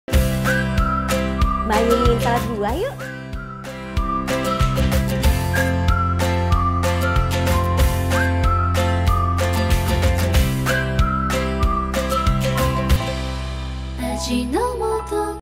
Resep Salad Buah Cocktail ala Mayumi® - 15s
Setelah seharian beraktivitas enaknya sih menikmati makanan atau minuman yang segar. Mau kan, Ma? Kalau gitu, ayo buat aja Salad Buah Cocktail ala Mayumi®!

Tonton video resepnya sampai habis supaya bisa nanti bisa langsung buat di rumah, ya!

Masih mau tau lebih banyak tentang kami?